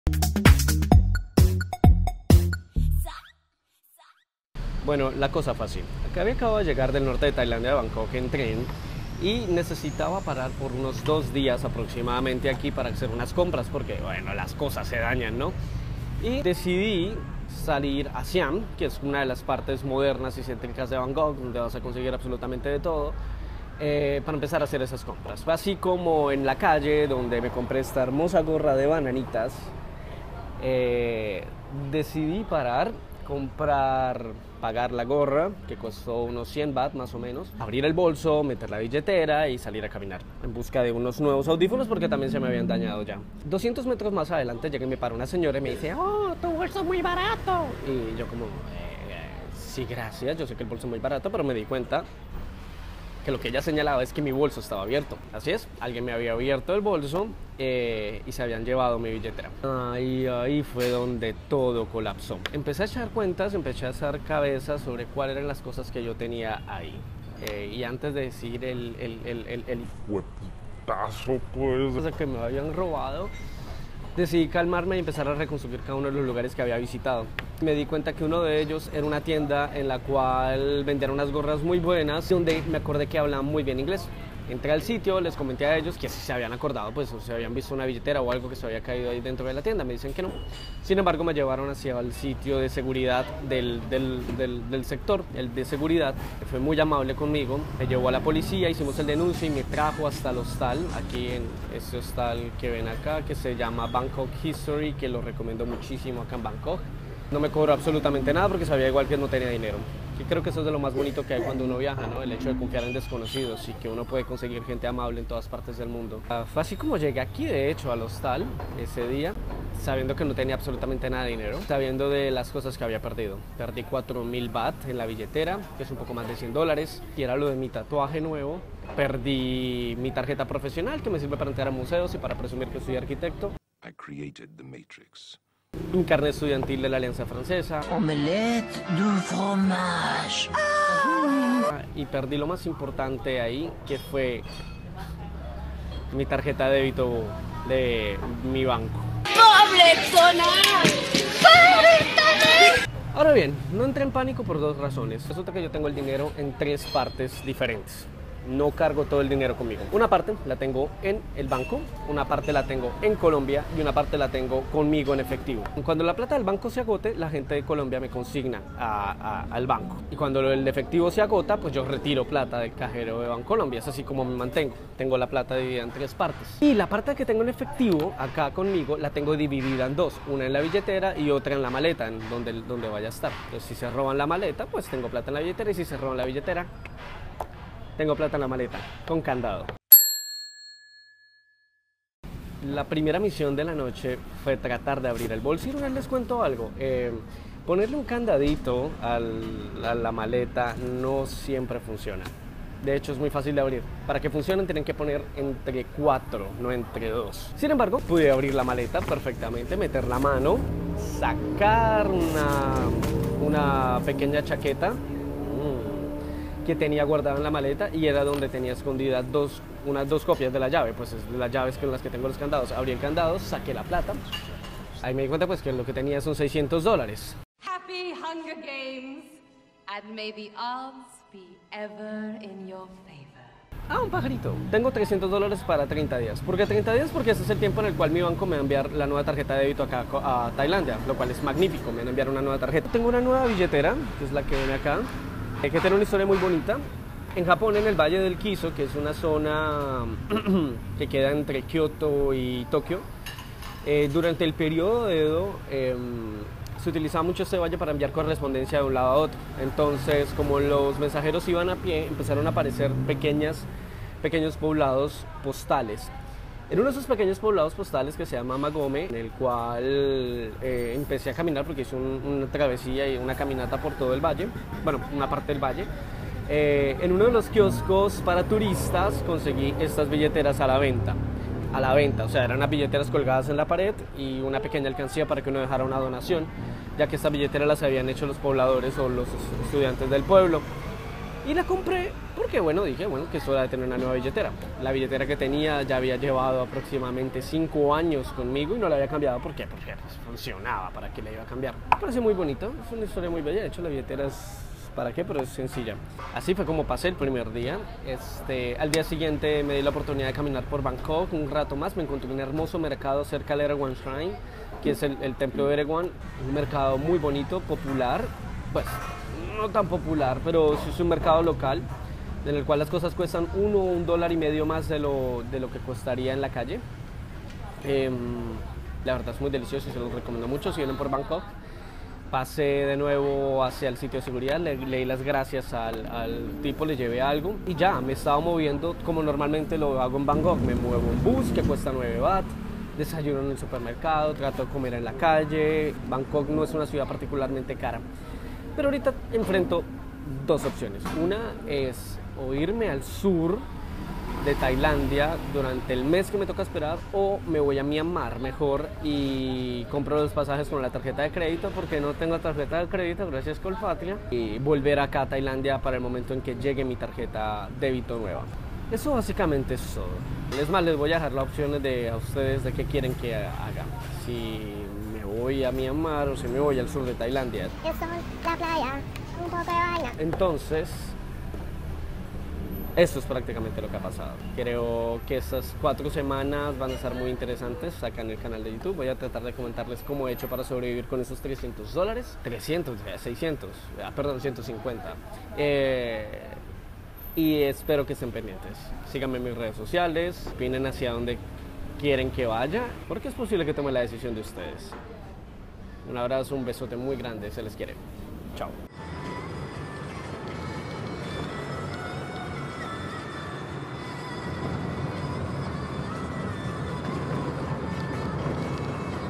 Después de un robo, ¡vivir un mes con 600 dólares!
Esta es la historia de mi robo en Bangkok y el inicio de todo un mes con sólo 600 dolares en el bolsillo. ¿Qué tanto puedo hacer?

¡Y PENDIENTES DE TODAS LAS REDES! 😈